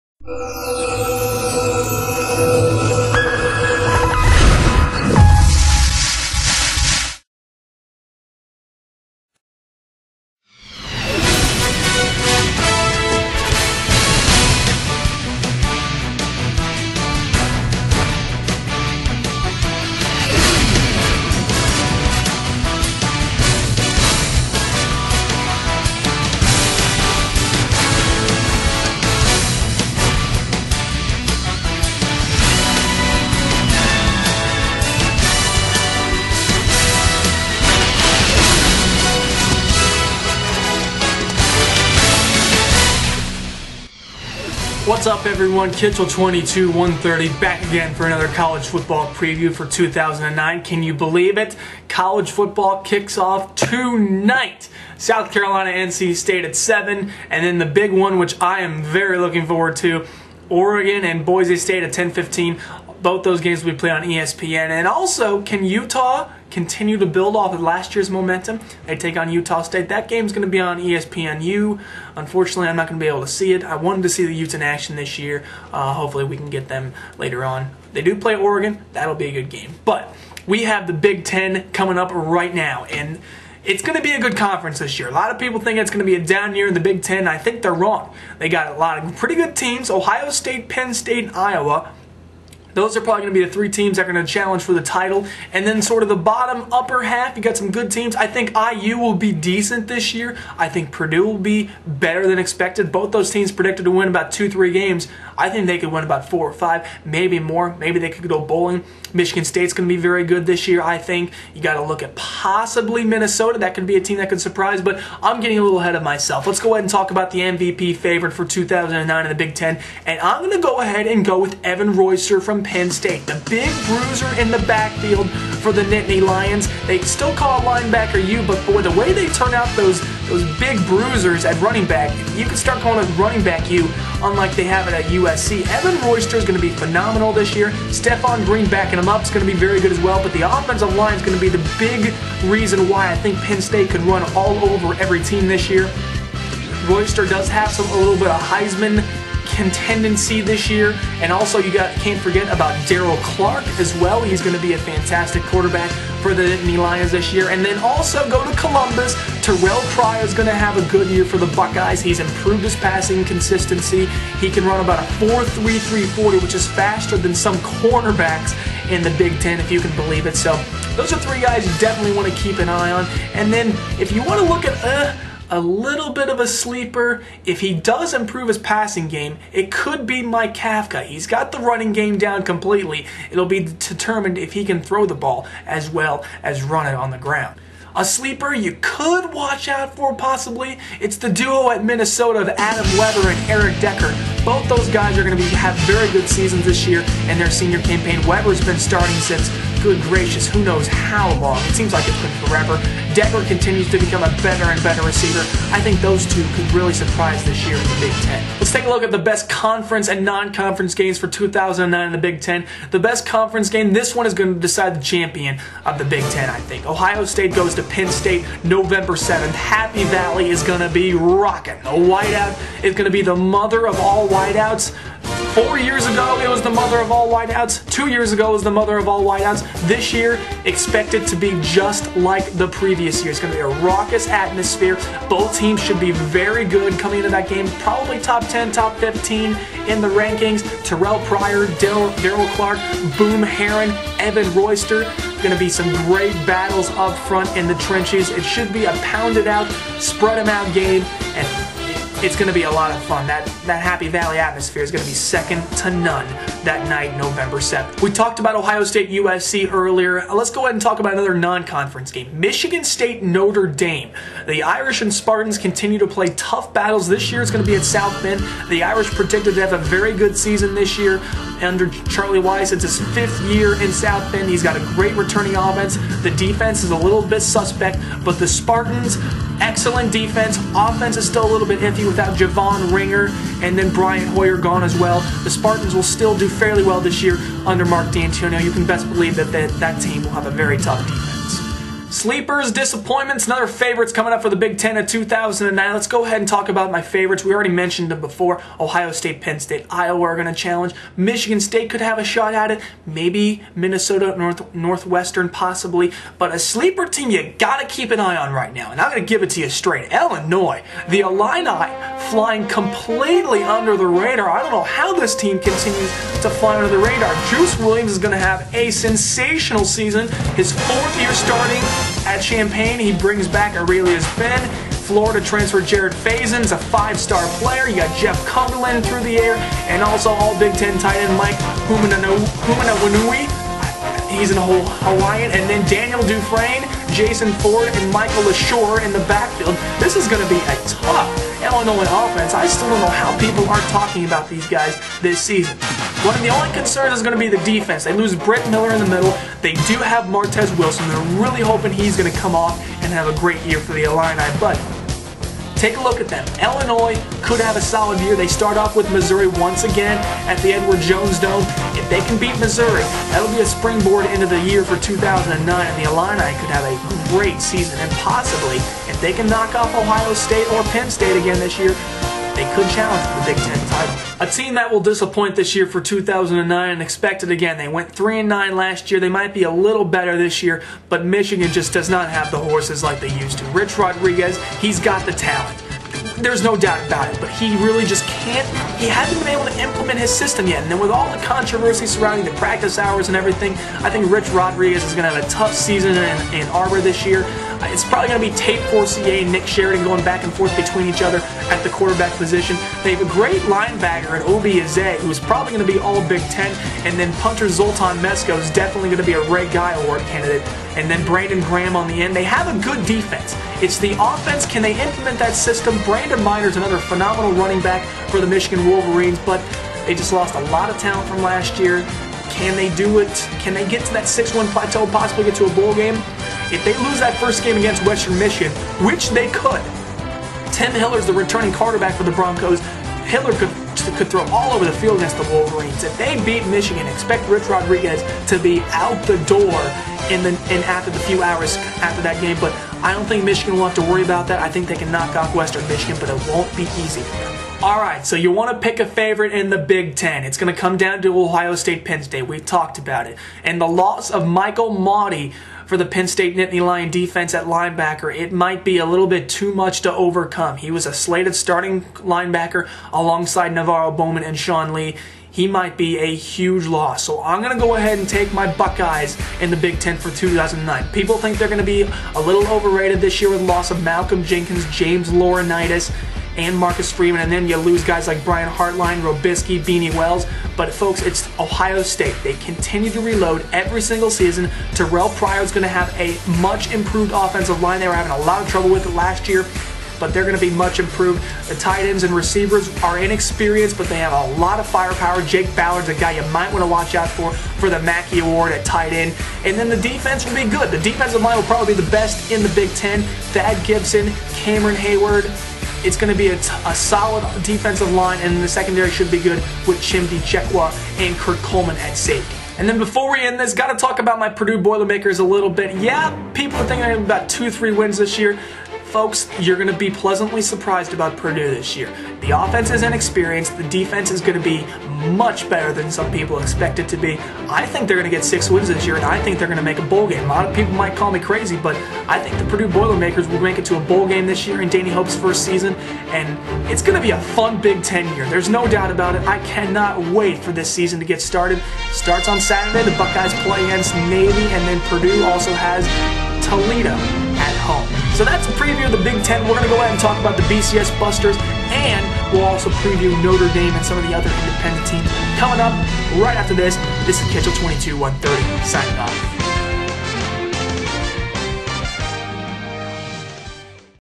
KSN 2009 College Football Previews - Big Ten
KITCHEL SPORTS NETWORK - The most anticipated preview is now here as the Big Ten will kick off tonight with Eastern Kentucky and Indiana. A lot of question marks on many of the teams in this conference this year including Ohio State and Penn State, both of which have to plug in new leaders on offense and defense. Will it be one of the big 2? Will Iowa break out? Can Michigan State rise to the occasion? Will Michigan overcome the accusations and succeed? Find out all of those answers here!